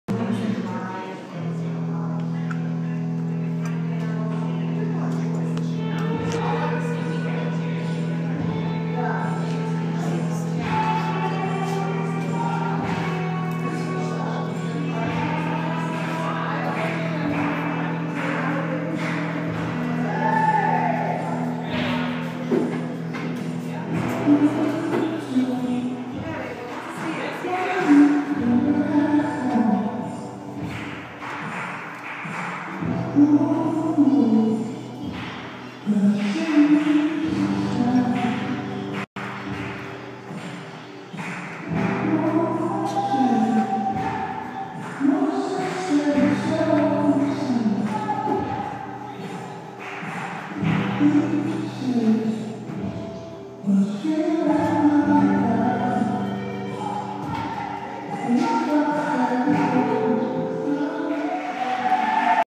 Matthew Fata contemporary
Amity dancing to a beautiful contemporary piece by Matthew Fata